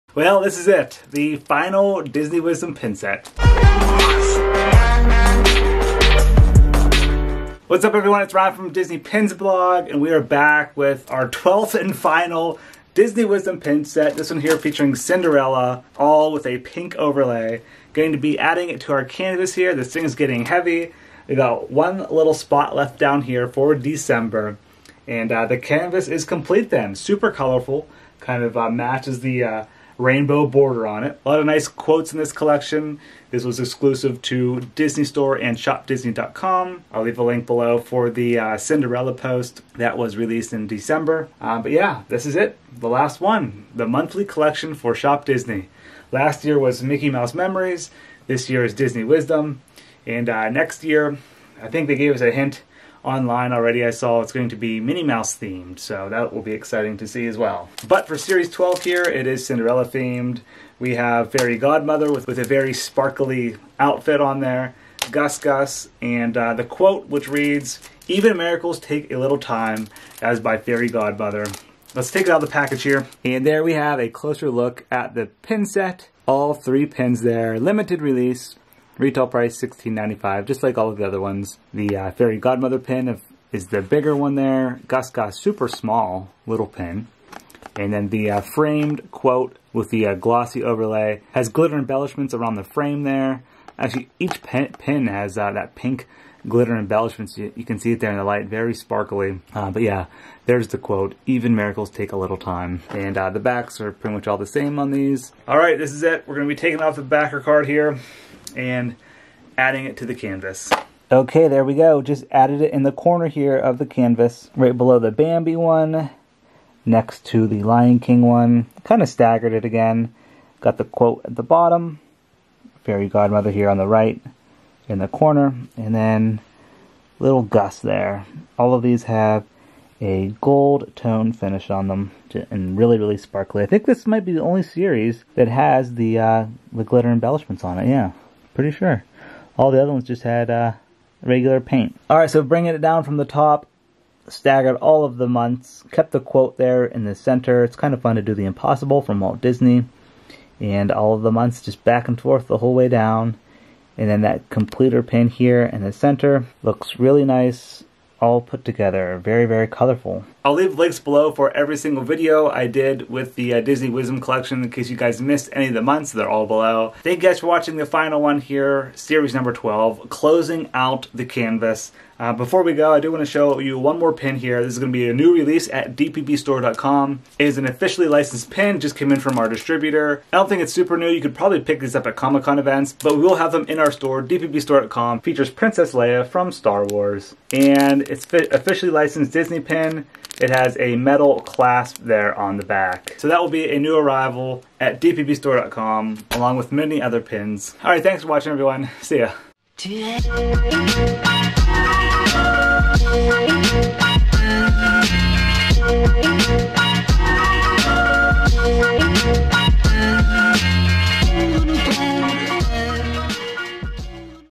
CINDERELLA DISNEY WISDOM PIN SET! | December 2019
Here is a look at the 12th and final release in the Disney Wisdom Collection at shopDisney and Disney Store! December 2019 features Cinderella.